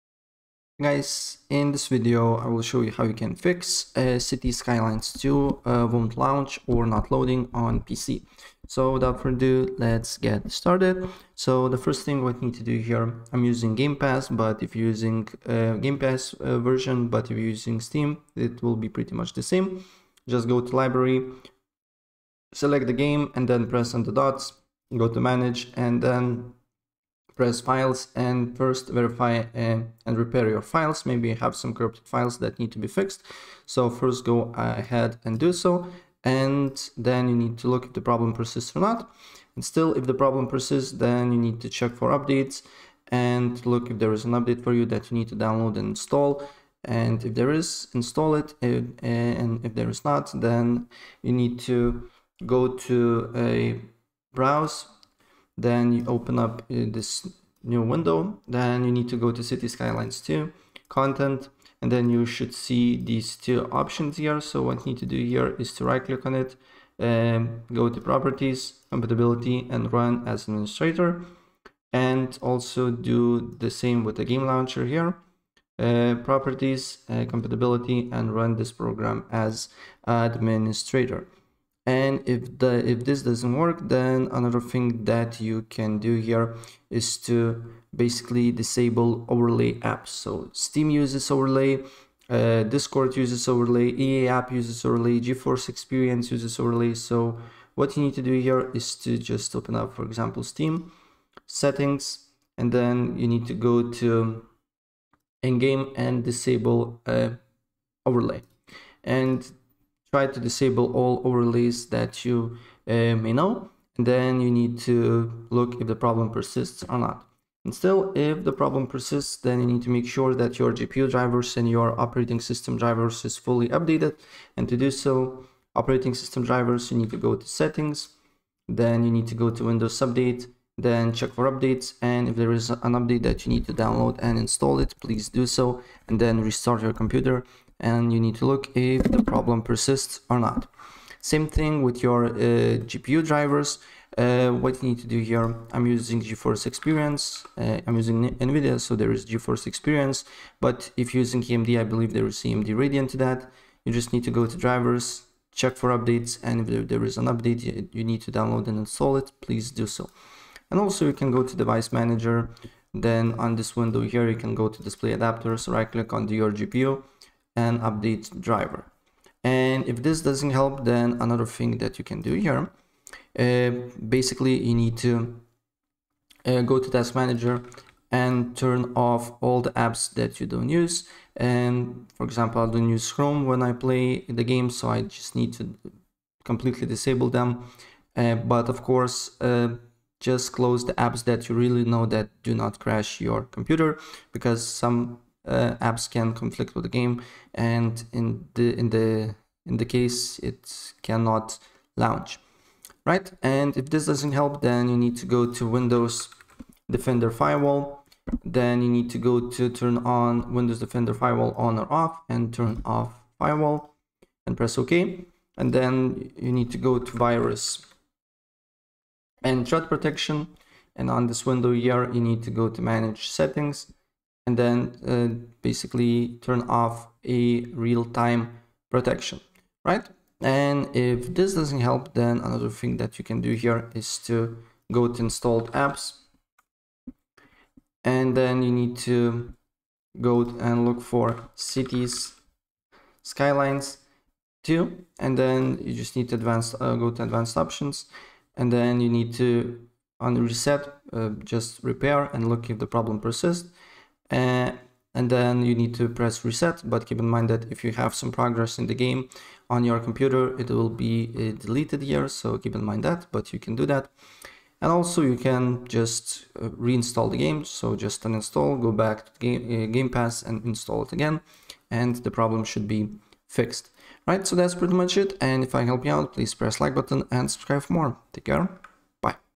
How To Fix Cities Skylines 2 Won't Launch or Load on Xbox Gamepass
In this video I will show you How To Fix Cities Skylines 2 Won't Launch or Load on Xbox Gamepass It's really easy and it will take you less than a minute to do it!